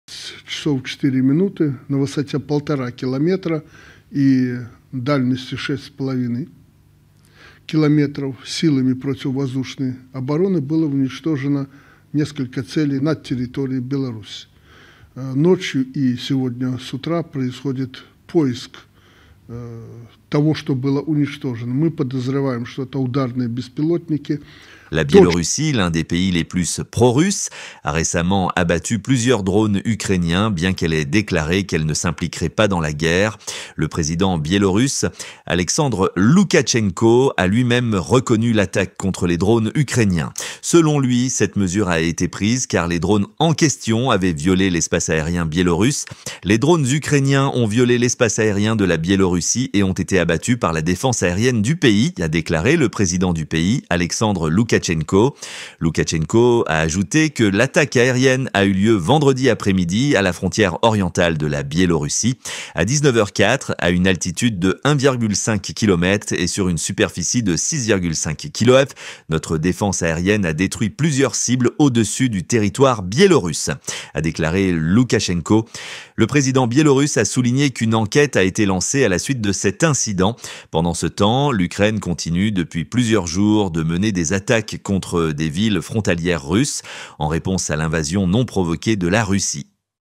La Biélorussie abat des drones ukrainiens : Loukachenko - "Ils ont violé notre espace aérien"
La Biélorussie, l'un des pays les plus prorusses, a récemment abattu plusieurs drones ukrainiens, bien qu'elle ait déclaré qu'elle ne s'impliquerait pas dans la guerre.